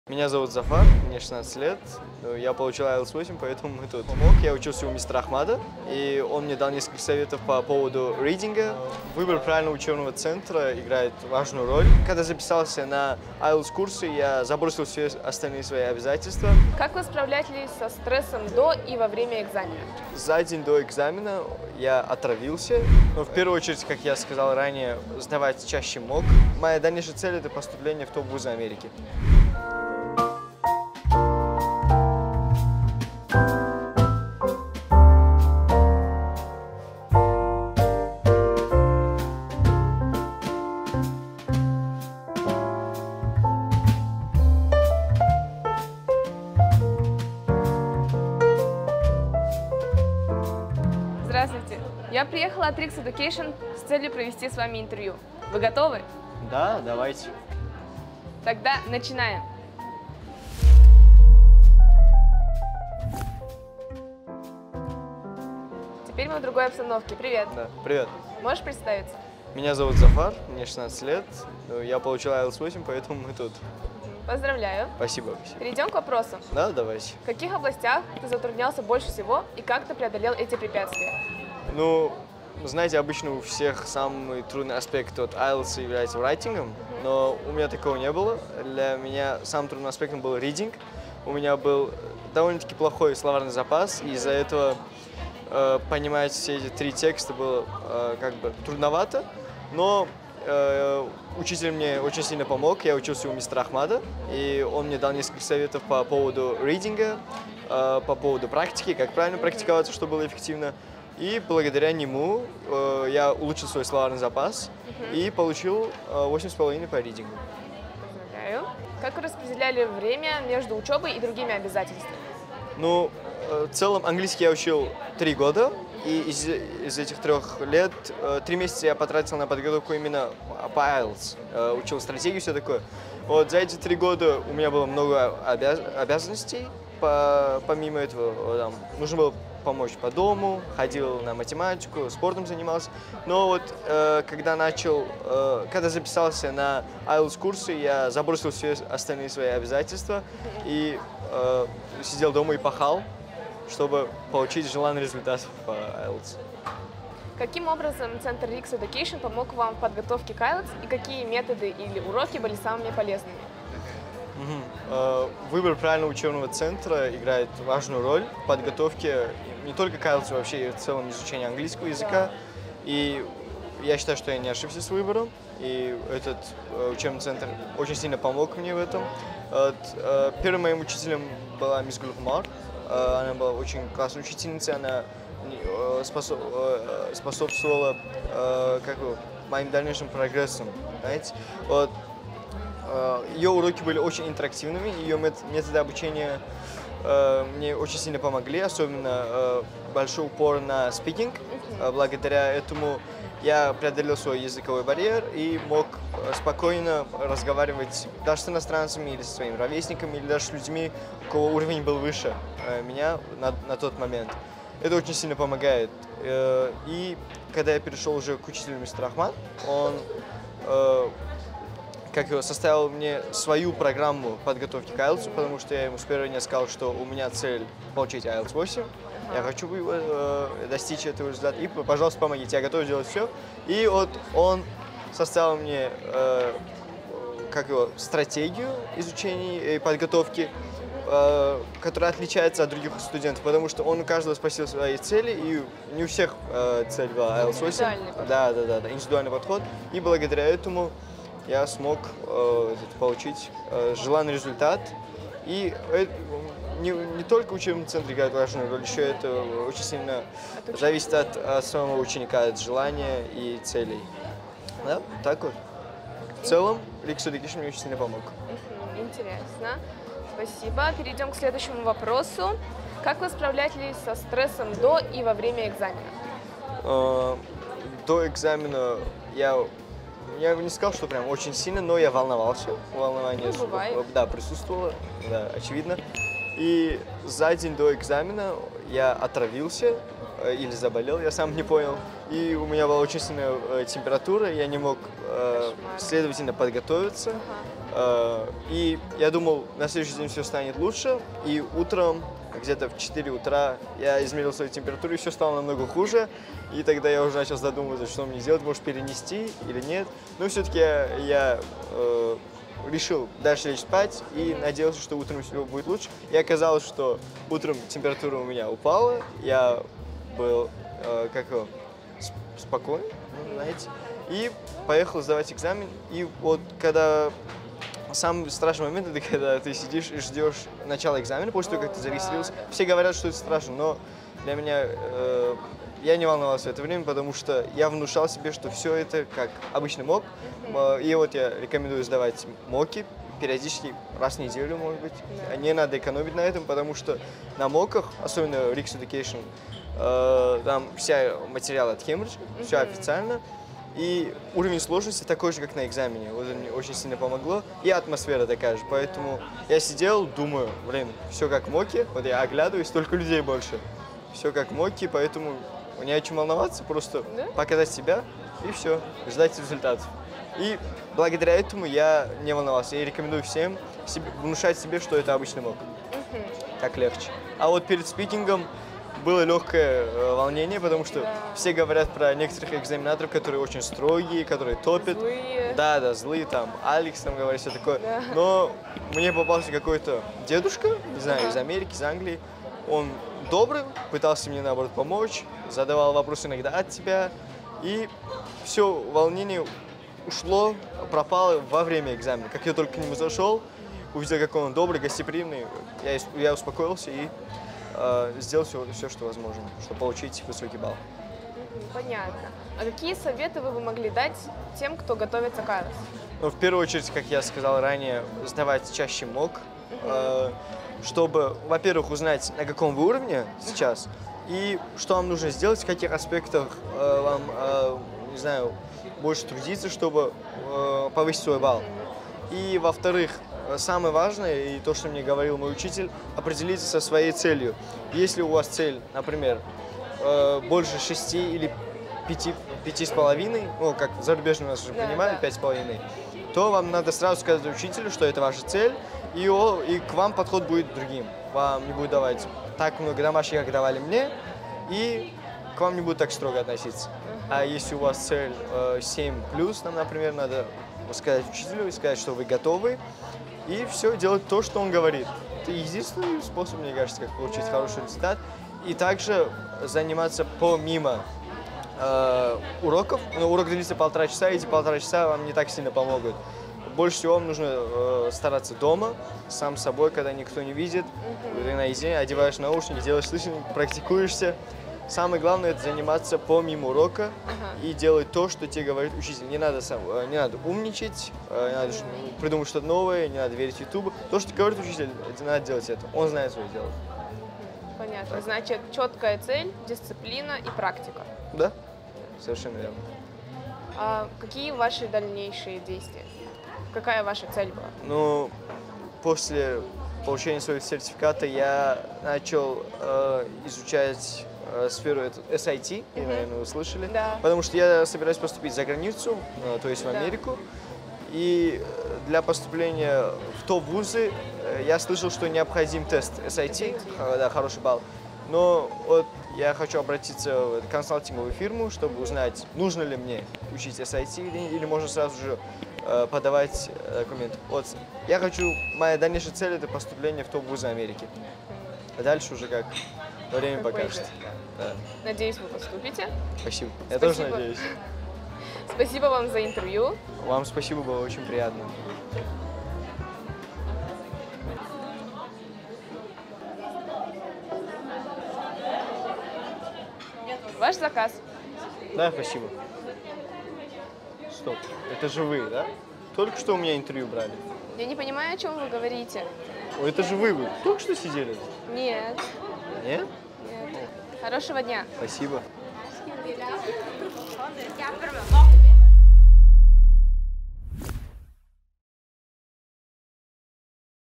IELTS 8.0 в 16 лет. Как получить Listening 9.0?
🌟 RIKS Education ning 16 yoshli o’quvchisi Zafar bilan ilhomlantiruvchi intervyu! 🎉

🎞 O'quv markazimizning eng so'nggi yangiliklaridan xabardor bo'lish uchun kanalimizga obuna bo'ling. Biz birgalikda eng yaxshi imkoniyatlar va yutuqlar kelajagini yaratamiz! 🚀

🌟 Вдохновляющее интервью с Зафаром, 16-летним учеником RIKS Education! 🎉